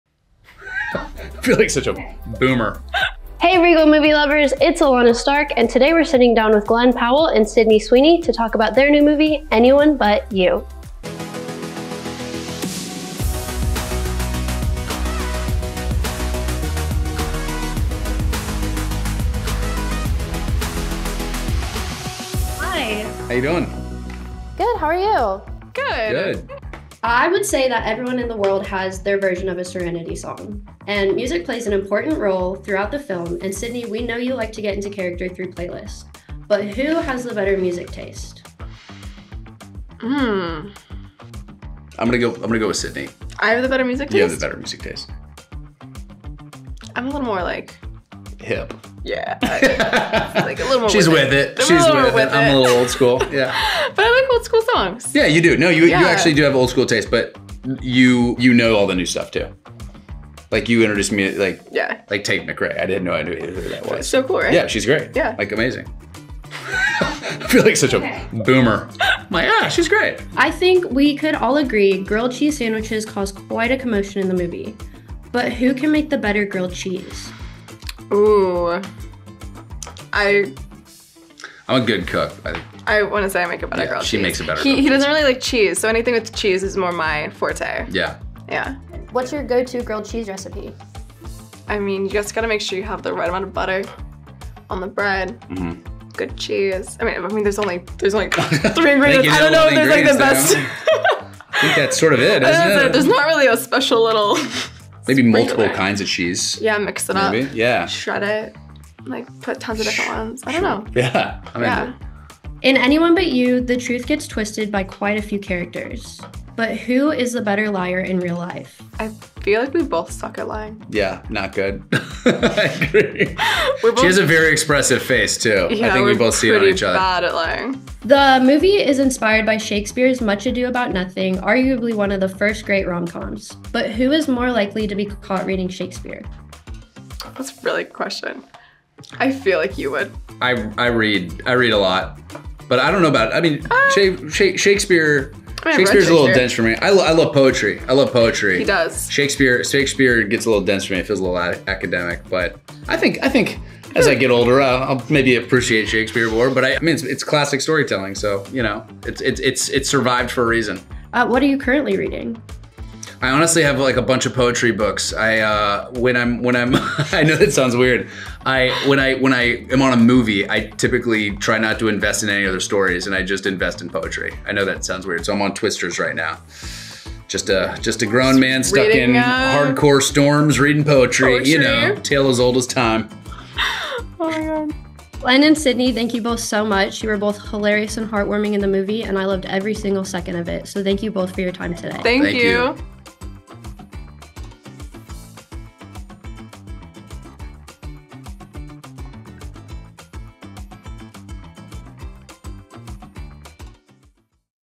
Sit Down with the Stars of Anyone But You (2023)
'Anyone But You' is coming to Regal theatres December 22.

In the edgy comedy 'Anyone But You,' Bea (Sydney Sweeney) and Ben (Glen Powell) look like the perfect couple, but after an amazing first date something happens that turns their fiery hot attraction ice cold — until they find themselves unexpectedly thrust together at a destination wedding in Australia. So they do what any two mature adults would do: pretend to be a couple.

Writers: Will Gluck, Ilana Wolpert
Stars: Dermot Mulroney, Alexandra Shipp, Sydney Sweeney, Glen Powell, Darren Barnet, Michelle Hurd